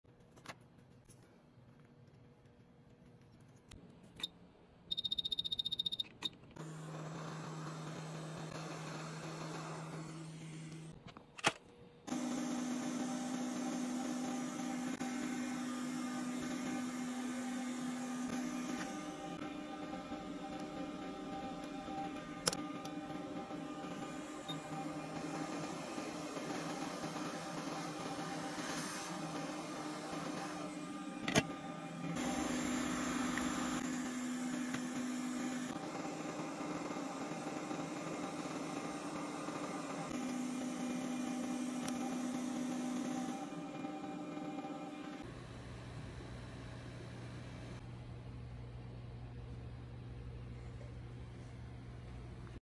CHIP SWAP ON A FAILED HDD PCB
This seagate HDD failed due to a bad PCB. Only swapping the board was an option here.